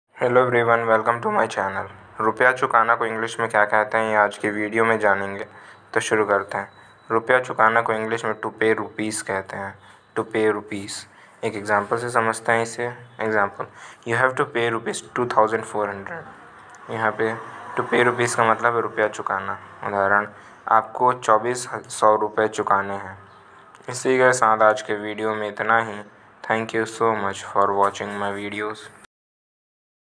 Rupaya chukana ko english mein kya kahate hain |रूपया चुकाना को इंग्लिश में क्या कहते हैं |To pay Rs
Rupaya chukana ko English me kya kahate hain
Rupaya chukana को इंग्लिश में क्या कहते है

रूपया चुकाना को इंग्लिश में to pay rs. कहते हैं
Rupaya chukana ko English mein to pay rs. kahate hain